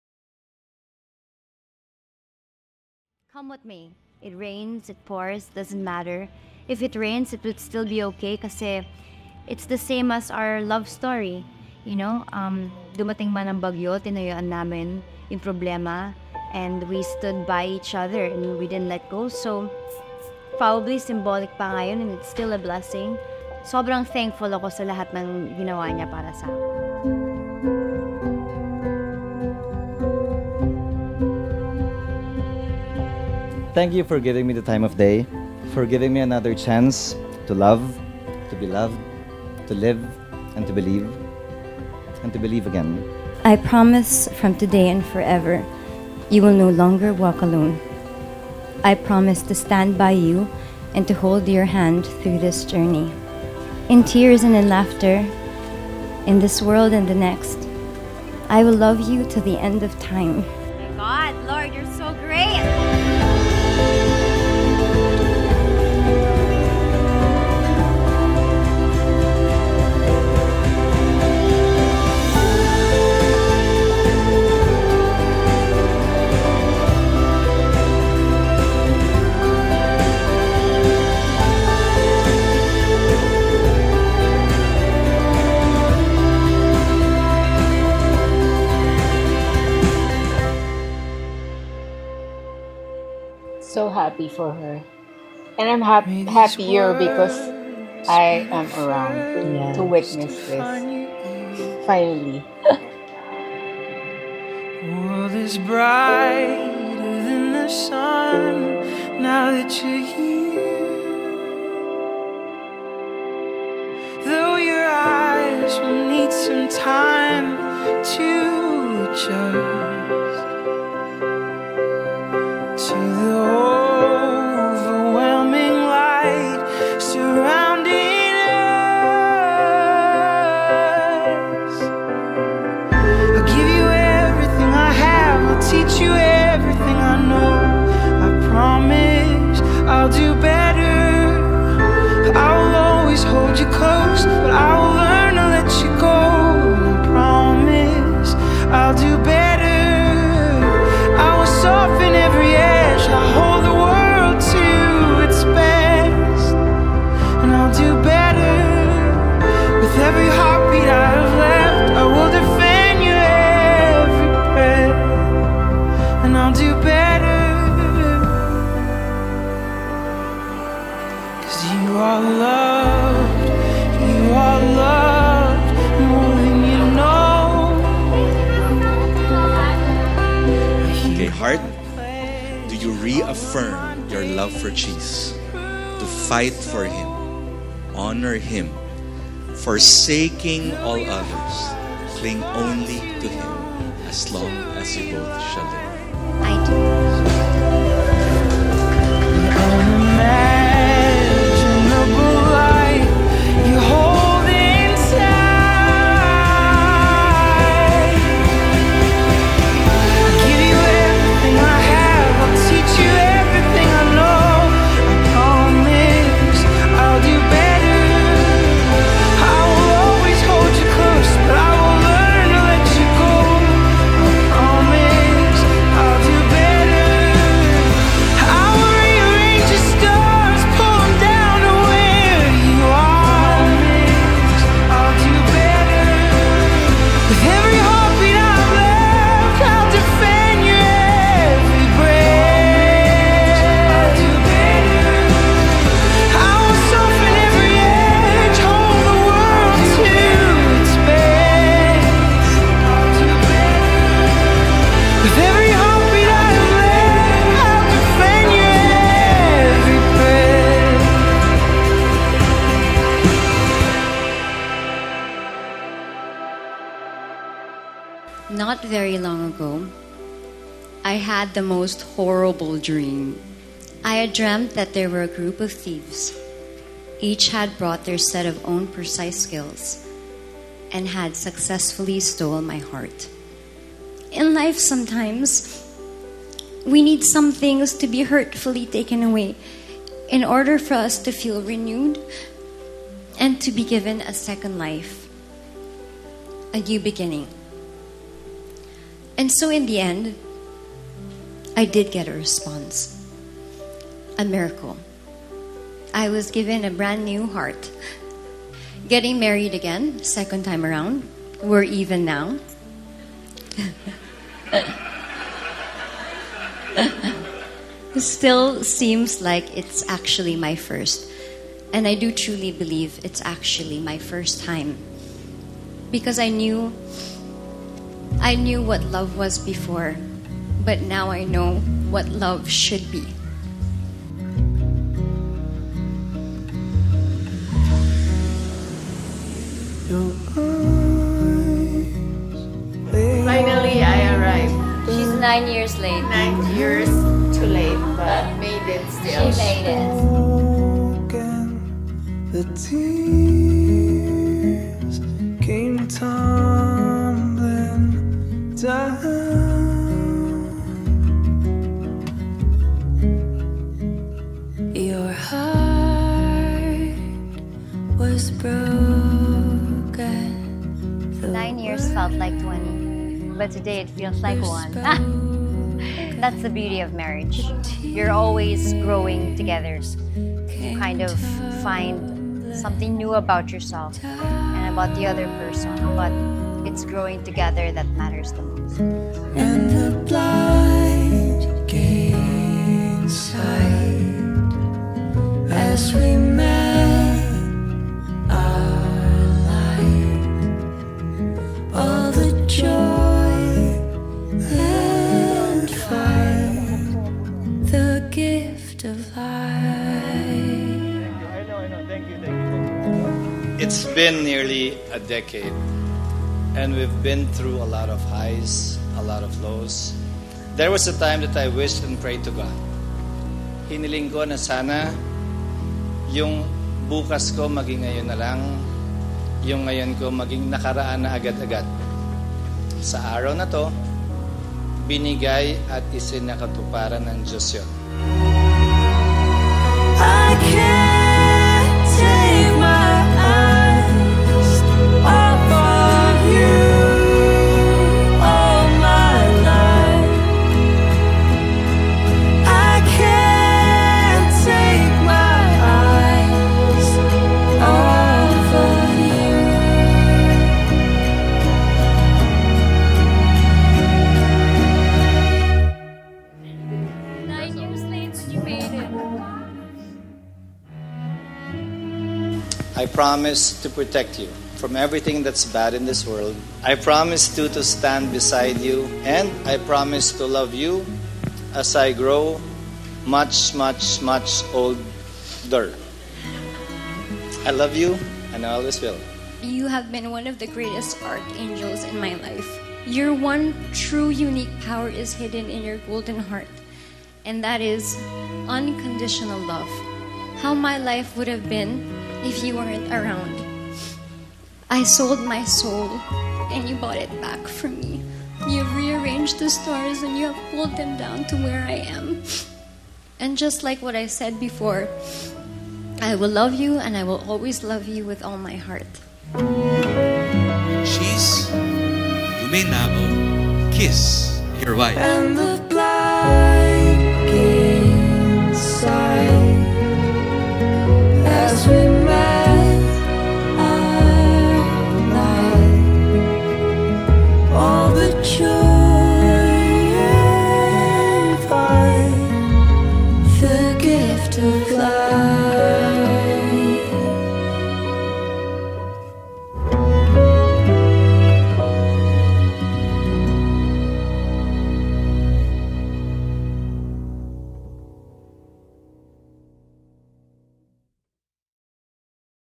Heart Evangelista and Chiz Escudero renew their vows! | The Renewal of Vows
Bob Nicolas makes his magic work with this same-day edit video of Heart Evangelista and Chiz Escudero's renewal of vows. Watch this video.